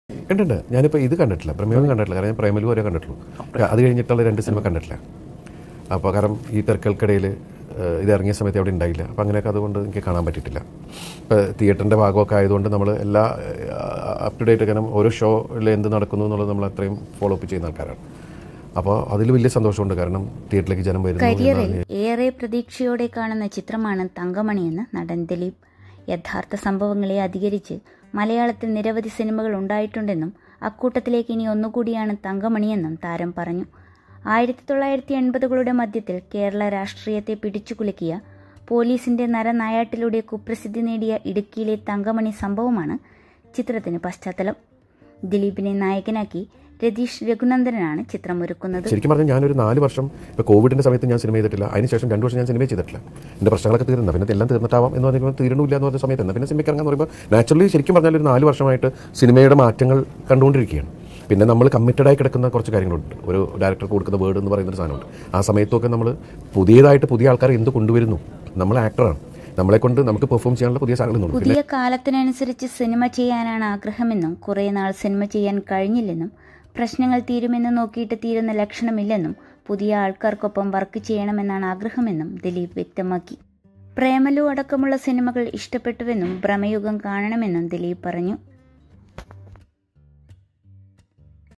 കുറെ നാള്‍ സിനിമ ചെയ്യാന്‍ കഴിഞ്ഞില്ല, പുതിയ കാലത്തിനനുസരിച്ച് സിനിമ ചെയ്യാനാണ് ആഗ്രഹമെന്നും ദിലീപ്
thankamani-movie-interview.